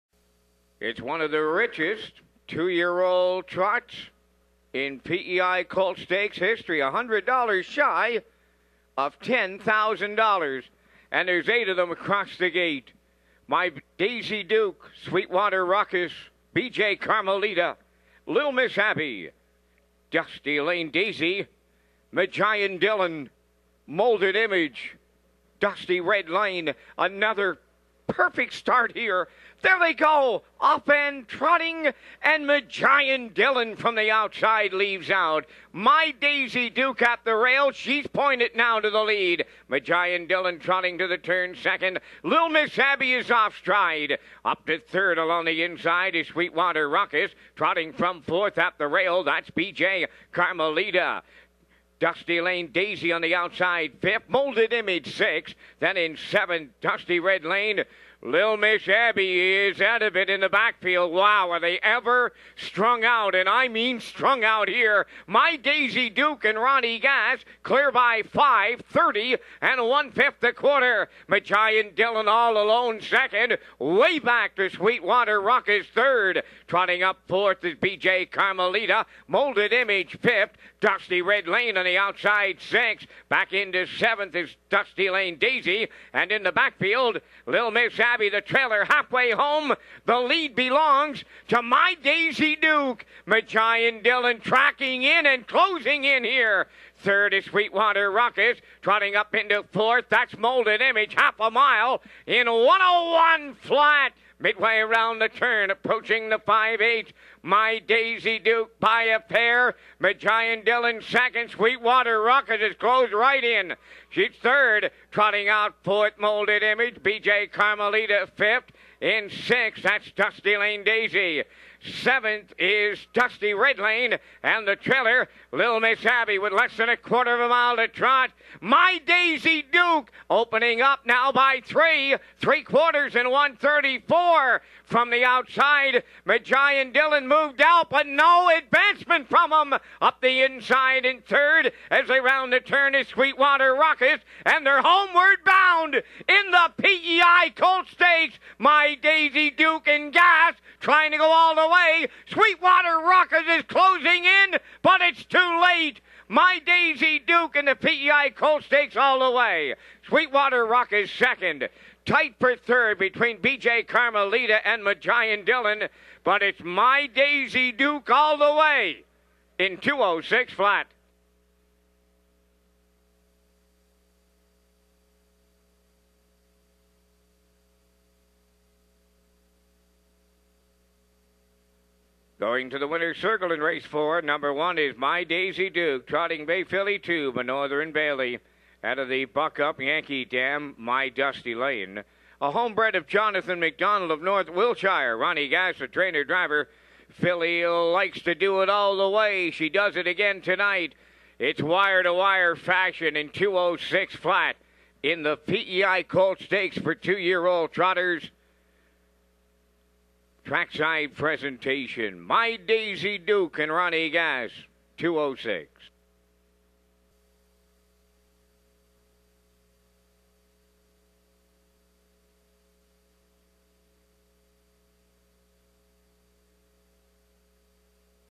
2011-09-15 R4 CDPEC PEI Colt Stakes 2 Year Old Trot My Daisy Duke
Contrary to the name of the stake -- the PEI Colt Stakes -- a filly took home the hardware in the lone division for the two-year-olds earlier on the card.

My Daisy Duke front-stepped to her 2:06 triumph in that tilt for trainer/driver Ronnie Gass. The daughter of Northern Bailey-Dusty Lane Buck led through panels of :30.1, 1:01 and 1:34 before using a :32-second final quarter to seal the deal by 1-1/2 lengths. Sweetwater Rukkus was next best, with Majian Dillion taking home the show dough.

Jonathan MacDonald of North Wiltshire, PEI bred and owns the rookie, who now boasts a 3-3-1 record in nine tries. The $4,455 payday bumped her earnings to $20,514.